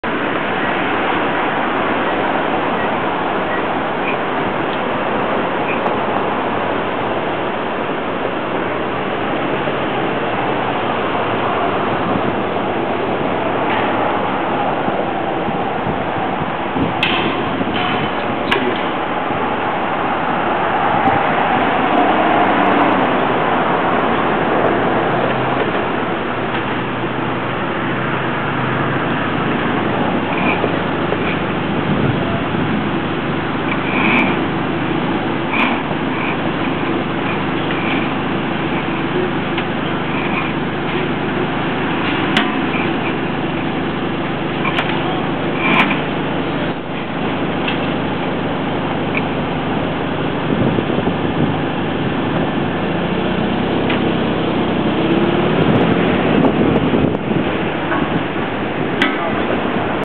hooking straps on quad rig to crane.MOV
Art Museum of Western Virginia, quad string of steel beams are rigged to the crane hook